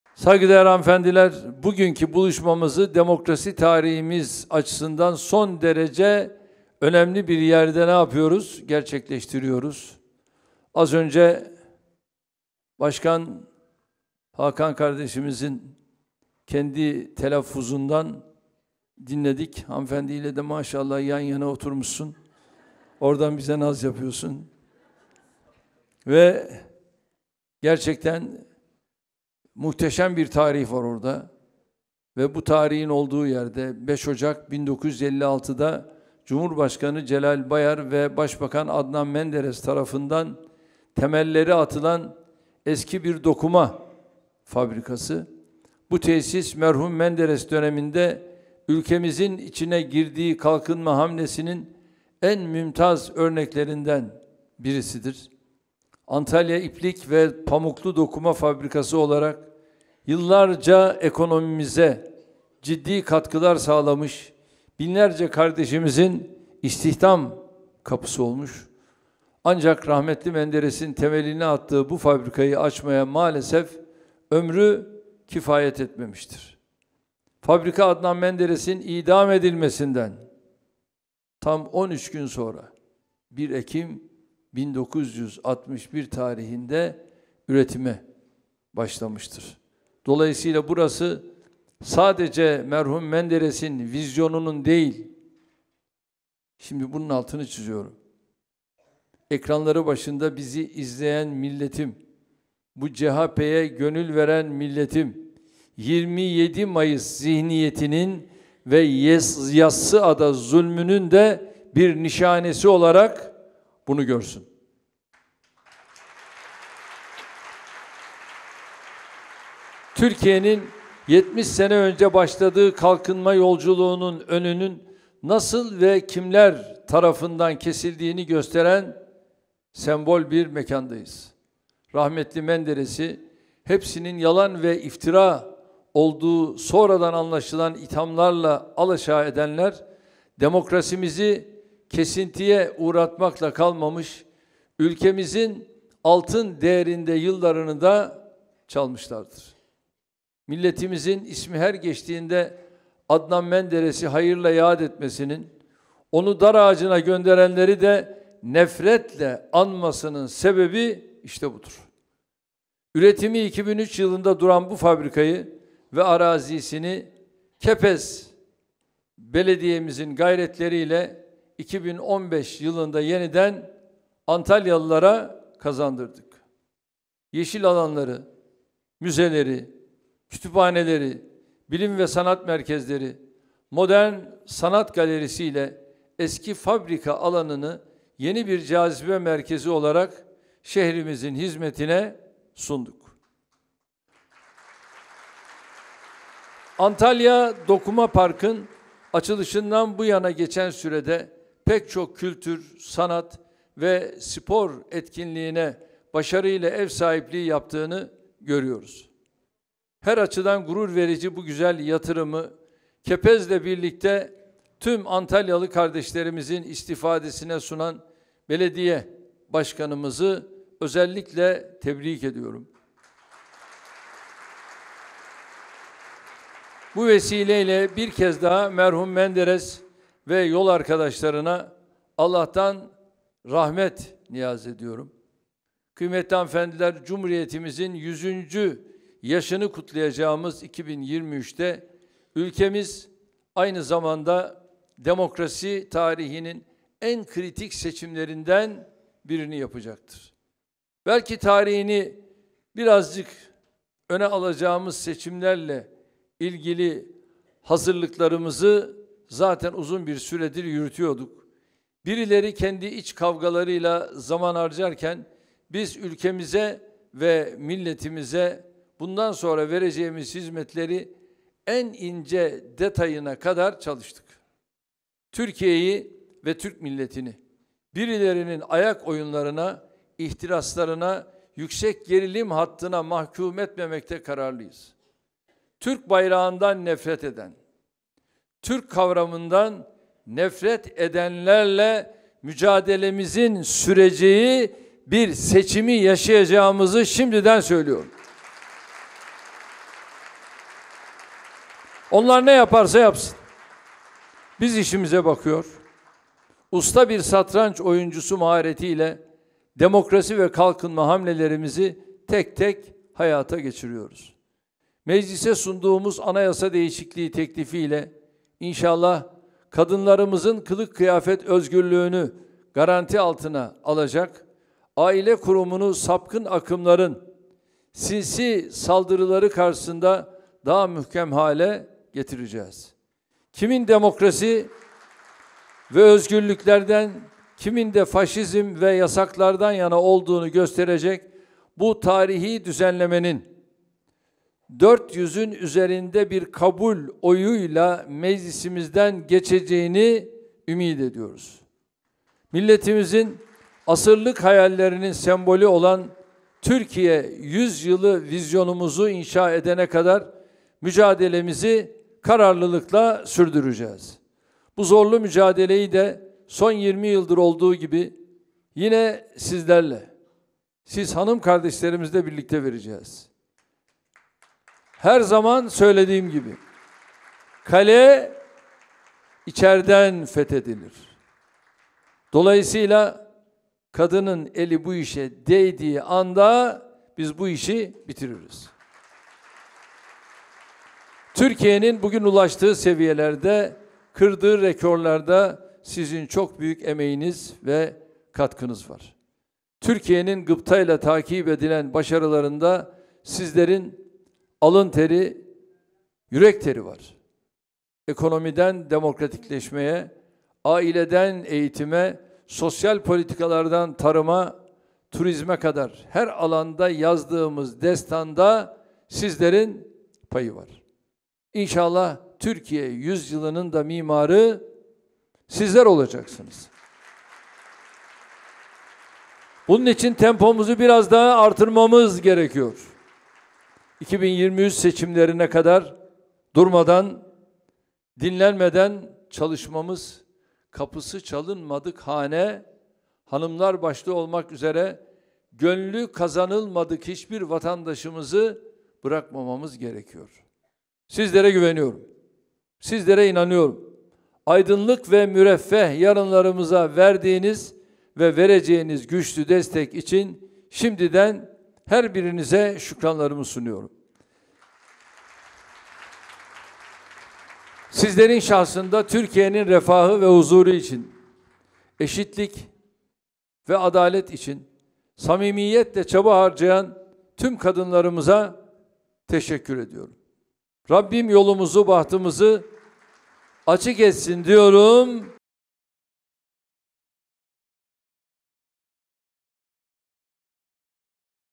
Erdoğan ve Ali Babacan arasında gerilim yaratacak sözler!
AK Parti Genel Başkanı ve Cumhurbaşkanı Recep Tayyip Erdoğan, Antalya Dokumapark Modern Sanatlar Galerisi'nde düzenlenen "Kadınlarla Büyük Türkiye Yolunda; Bilim, Kültür, Sanat, Spor ve Siyasette Kadın Buluşması Programı"na katıldı. Erdoğan, DEVA Partisi Genel Başkanı Ali Babacan ve ekibinin "Devrim Kanunları" çıkışıyla ilgili eleştirilerde bulundu.